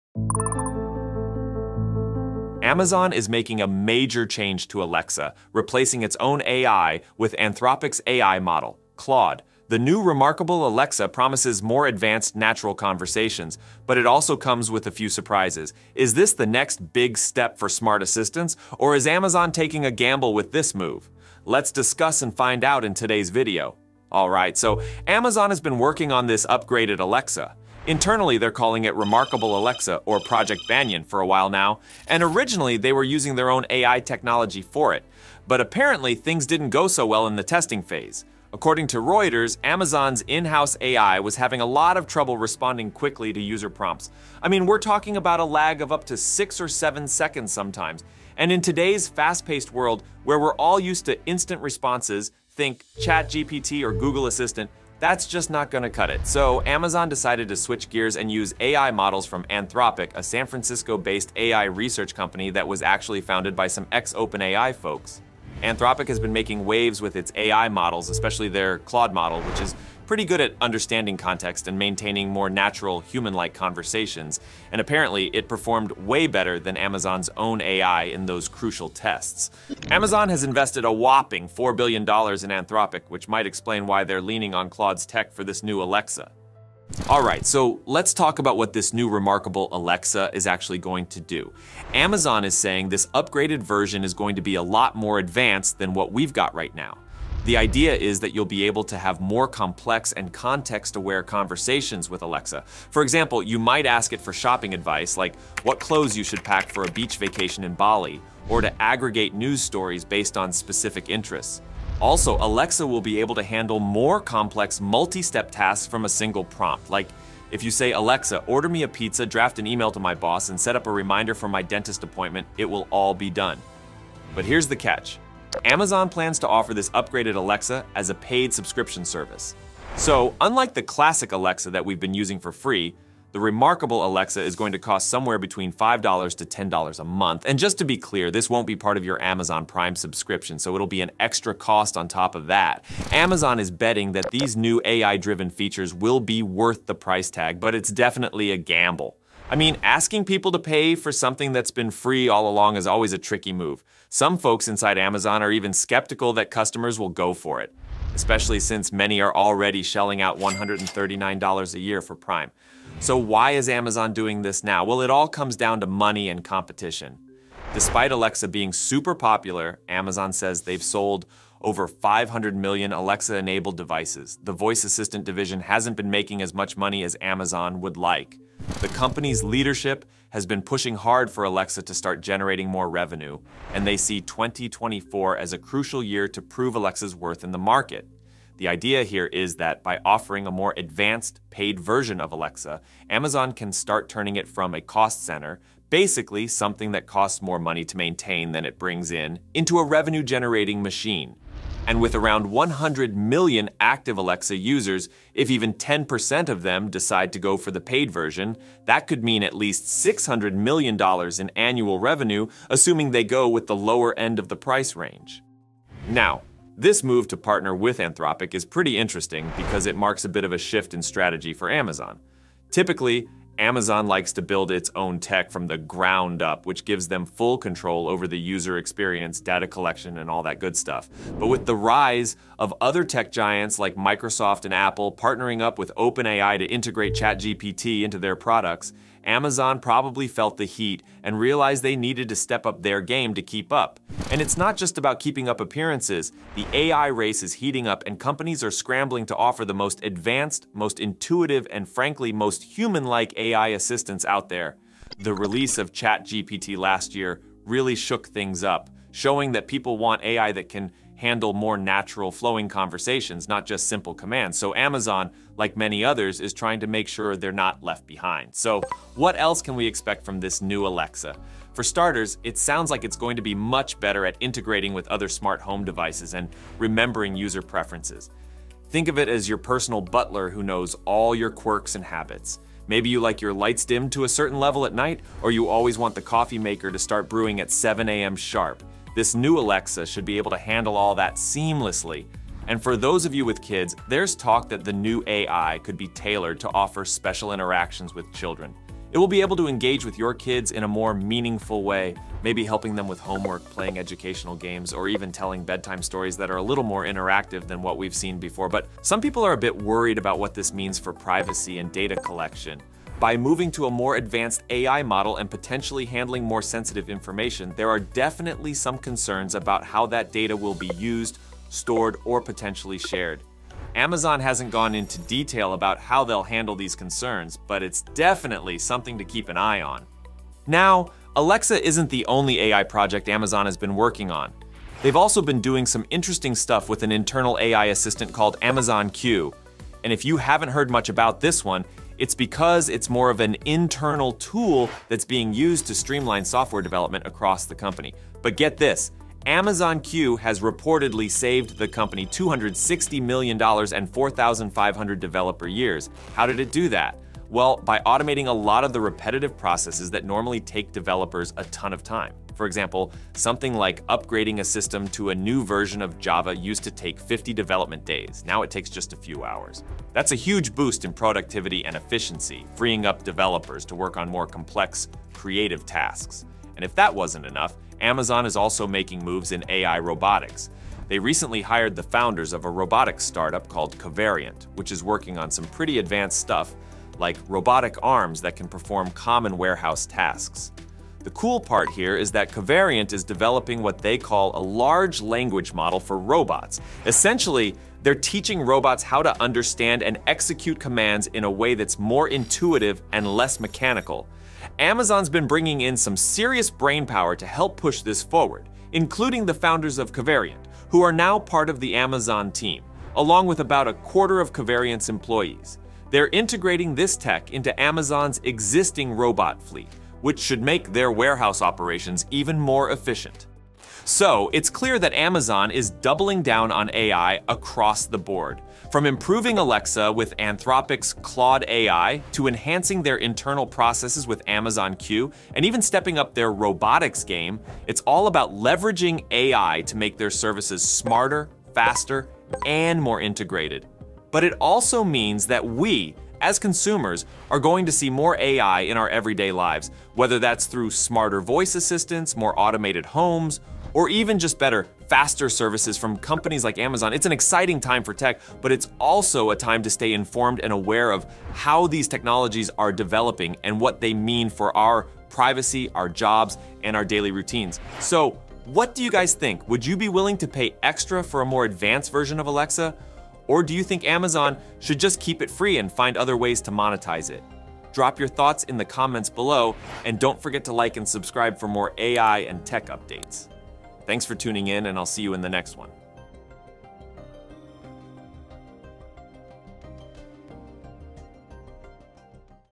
Alexa Is Now Claude AI
Amazon is upgrading Alexa by replacing its own AI with Anthropic's Claude model, creating a more advanced voice assistant called "Remarkable Alexa." This new version promises natural, context-aware conversations and multi-step task handling, but it will come as a paid subscription service. Amazon aims to generate more revenue with this move, betting that the enhanced capabilities of the new Alexa will attract users willing to pay for improved features.

🔍 Key Topics Covered:

Amazon is transforming Alexa with a new AI model, Claude, from Anthropic, enhancing natural conversation capabilities.
The introduction of "Remarkable Alexa" offers more complex, context-aware interactions and multi-step task handling.
Amazon's strategy shift towards a subscription model aims to generate revenue while competing in the growing AI assistant market.

🎥 What You'll Learn:

How Amazon's partnership with Anthropic's Claude model aims to revolutionize the Alexa experience.
The potential benefits and drawbacks of "Remarkable Alexa," including its advanced features and subscription cost.
The strategic reasons behind Amazon's decision to shift away from its in-house AI technology and invest heavily in Anthropic.

📊 Why This Matters:

Amazon's move to enhance Alexa with more sophisticated AI reflects the competitive race in AI technology and smart assistants.
The decision to charge for the new version raises questions about consumer acceptance and the future direction of AI-driven services.
Understanding these changes can provide insight into the evolving landscape of digital assistants and the integration of advanced AI in daily life.